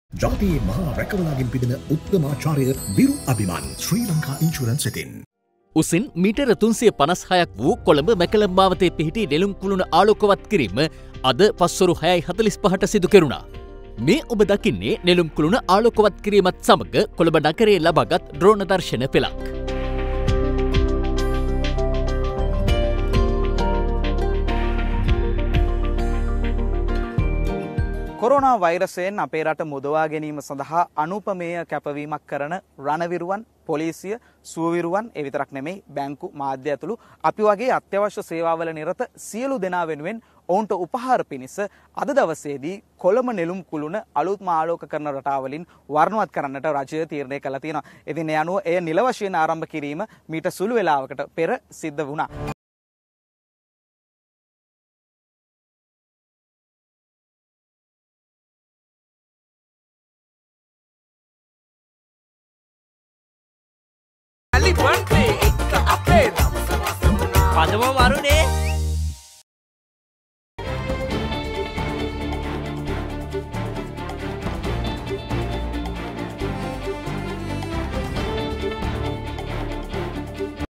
ආලෝකමත් වු නෙළුම් කුළුණ...
Lotus Tower to light up to salute those on COVID-19 front-line